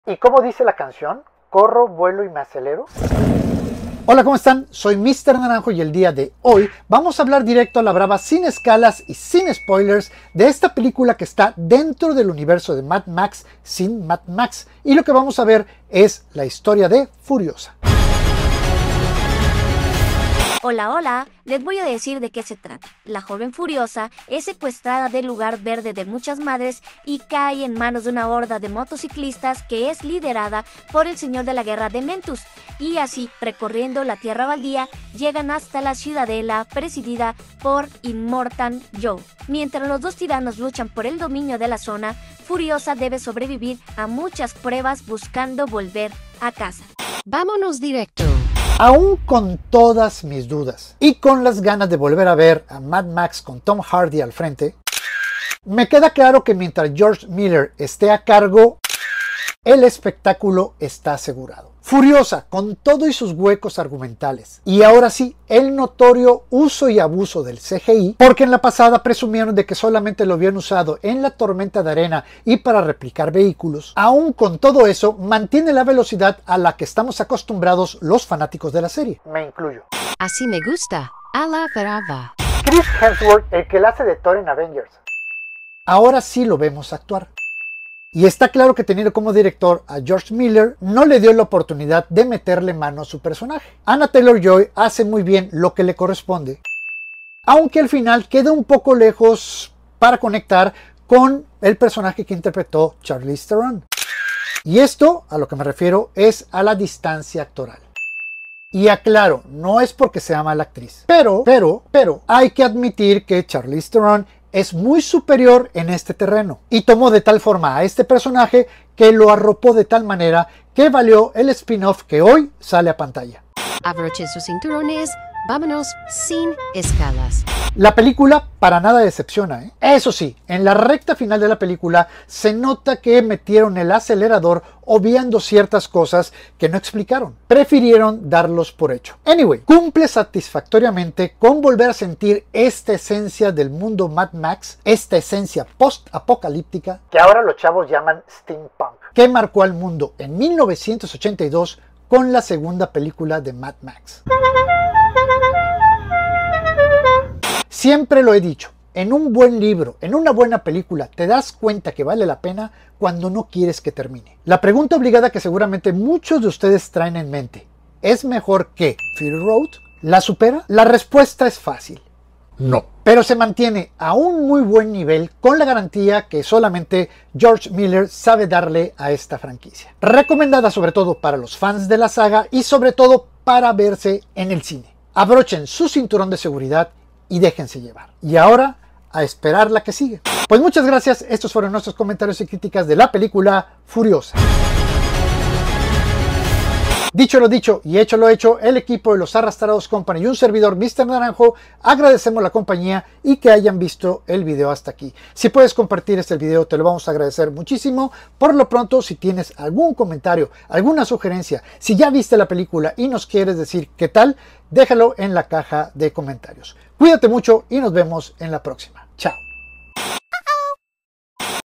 VIDEOCRITICA - FURIOSA
Sin Spoilers

De la saga de MAD MAX llega la nueva película de George Miller